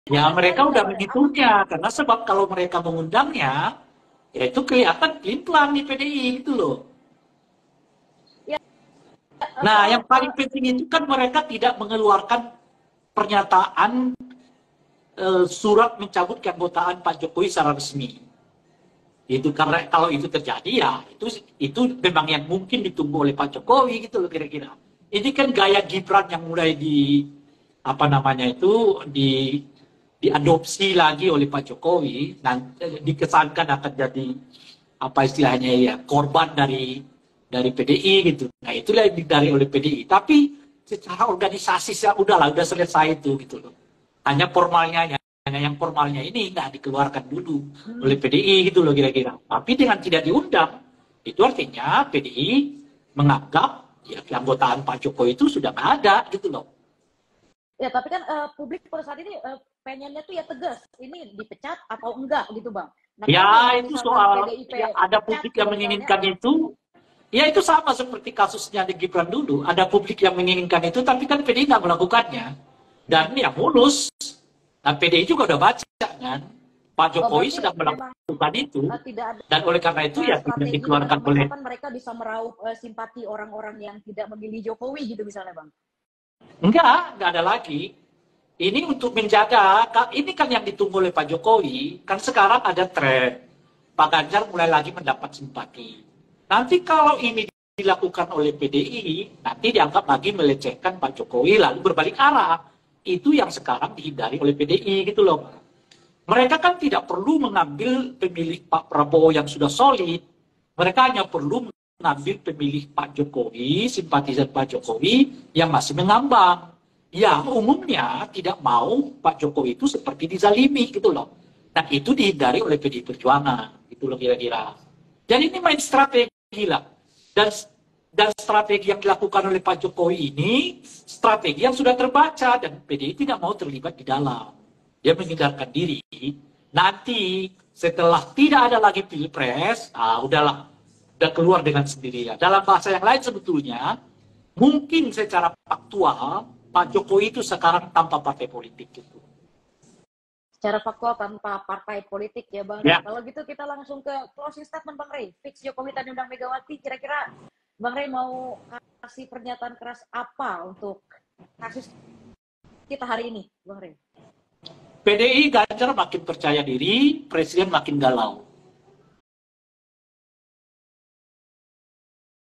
TAK DIUNDANG DI HUT PDIP, BAGAIMANA STATUS KEANGGOTAAN JOKOWI?
Pengamat Politik/ Direktur LIMA (Lingkar Madani) Ray Rangkuti mengatakan keanggotaan Joko Widodo (Jokowi) di PDIP sudah selesai. Hal tersebut terlihat dari Jokowi yang tidak diundang di Hari Ulang Tahun (HUT) ke-51 PDIP tanggal 10 Januari 2024.